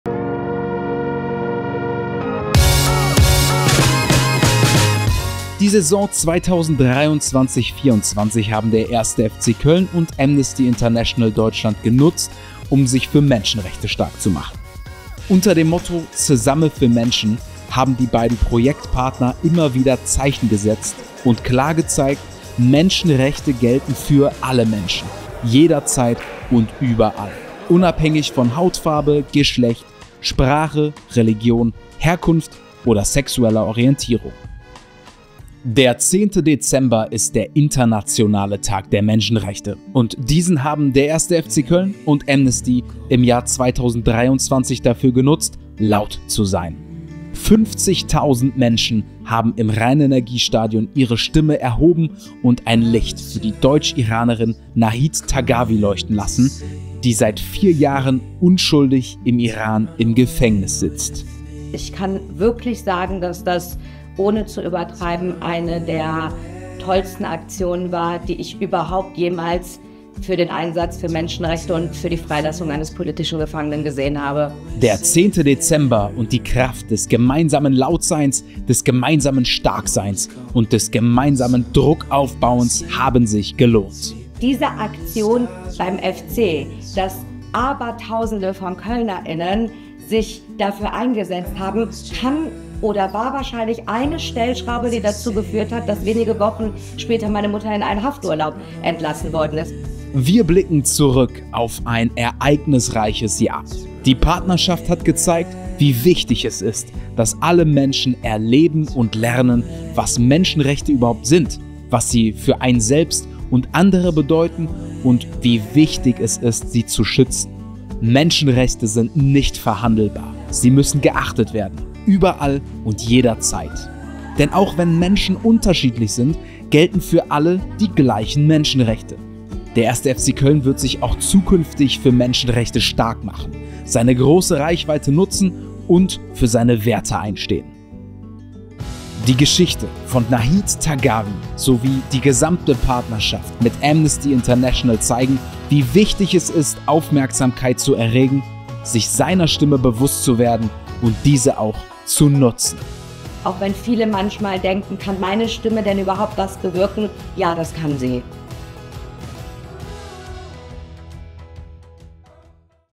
Bring Nahid zurück nach Köln!
Zusammen mit dem @fckoeln blicken wir zurück auf eine ereignisreiche Saison 2023/24. Unter dem Motto ZesammeFürMenschen haben wir gezeigt: Menschen sind nicht gleich, aber ihre Rechte sind es.

Gemeinsam machen wir uns für die Kölnerin Nahid Taghavi stark. Wir unterstützen ihre Tochter und  FC-Köln-Fan, Mariam Claren, dabei ihre Mutter aus der Haft im Iran freizubekommen.

Bring Nahid home!

Die Kölnerin Nahid Taghavi ist seit 2020 im berüchtigten iranischen Evin-Gefängnis inhaftiert.

Nahid Taghavi ist eine iranisch-deutsche Frauenrechtlerin. Seit ihrer Festnahme musste sie monatelang in Isolationshaft. Sie wurde gefoltert. In einem unfairen Gerichtsverfahren wurde sie wegen angeblicher Beteiligung an einer "illegalen Gruppe" und wegen "Propaganda gegen den Staat" zu zehn Jahren und acht Monaten Haft verurteilt.